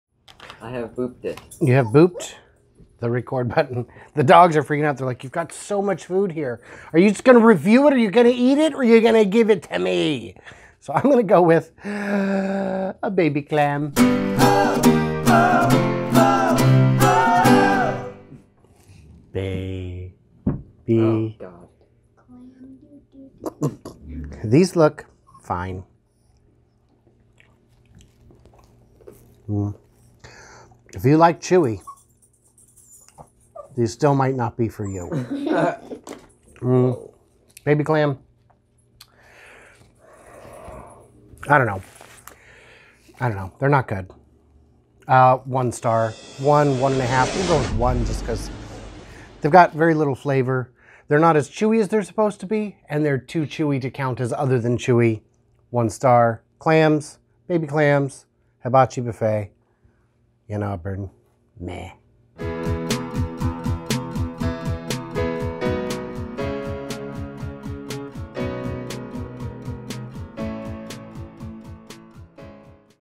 Baby clams at Hibachi Buffet are... just no
Baby clams in the shell is available at Hibachi Buffet in Auburn, Washington. It's a nice offering but it isn't great. Gotta really have a hankering for some clams to find joy in this menu item, but it is pretty premium for a budget buffet.

If you want to collaborate or get reviewed, get in touch.